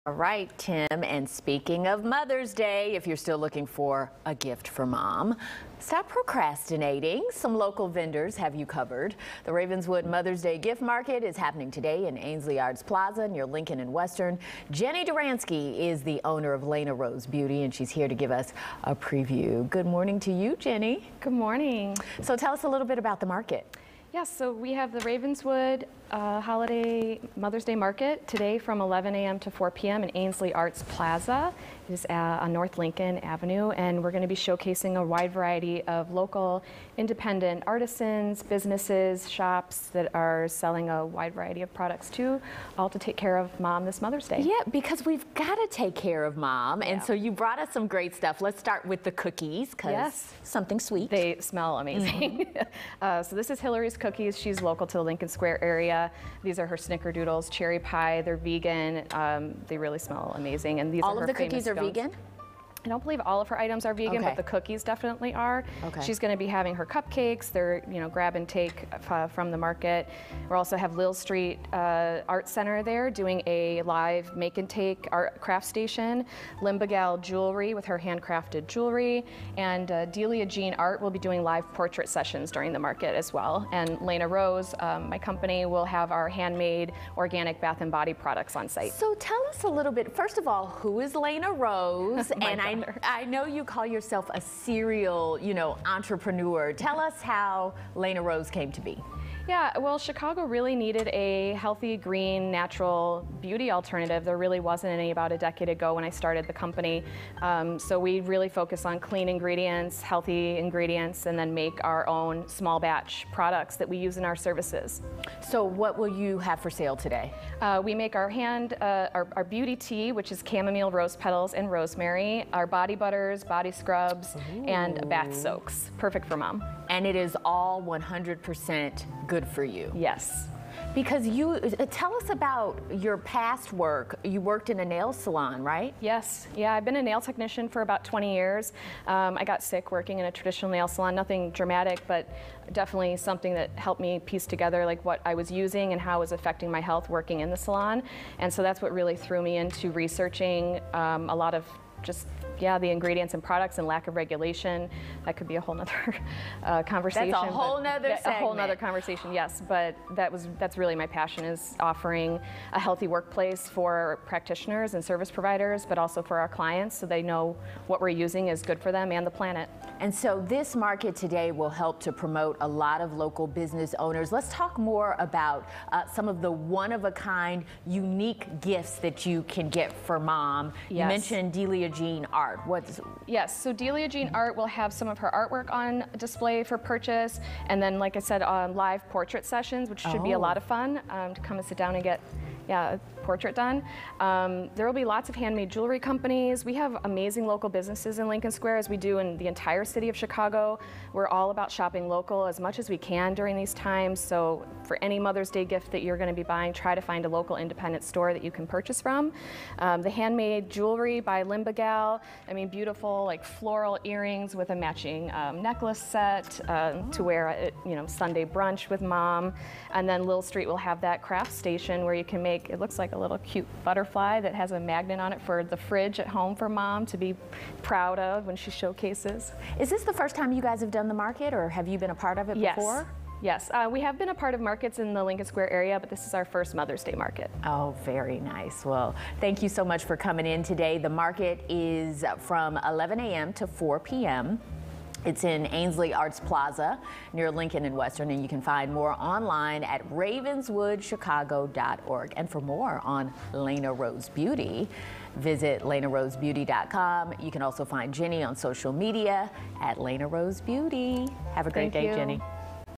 Local creators join for Ravenswood Mother's Day Gift Market
Jenny Duranski, founder of Lena Rose Beauty, joins us in studio to tell us about today's Ravenswood Mother's Day Gift Market, and other vendors to check out if you're still looking for a gift for mom.